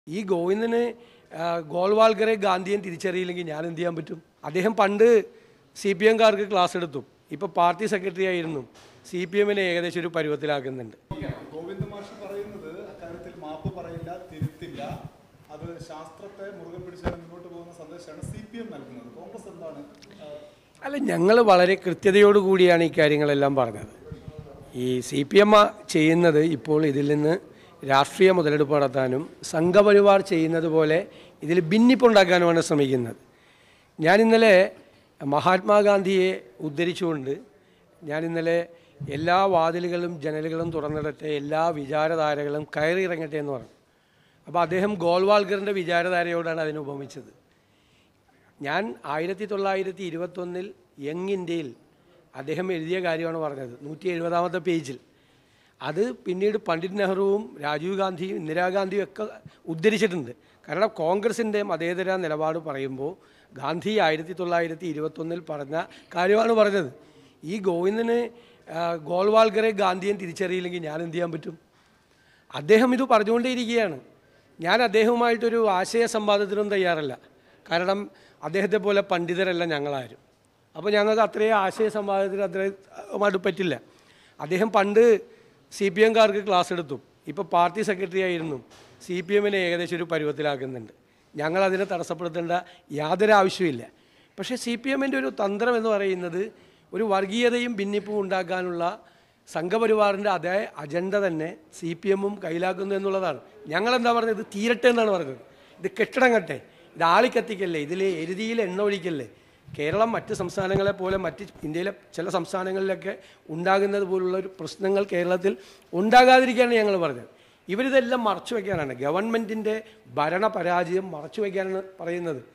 'അദ്ദേഹം പണ്ട് ക്ലാസെടുത്തു, ഇപ്പോൾ പാർട്ടി സെക്രട്ടറിയായിരുന്ന് CPMനെ ഒരു പരുവമാക്കുന്നുണ്ട്..'
അദ്ദേഹം പണ്ട് സിപിഎംകാർക്ക് ക്ലാസെടുത്തു, ഇപ്പോൾ പാർട്ടി സെക്രട്ടറിയായിരുന്ന് CPMനെ ഒരു പരുവമാക്കുന്നുണ്ട്..; എം വി ഗോവിന്ദന് മറുപടിയുമായി വി ഡി സതീശൻ

Happy viewing!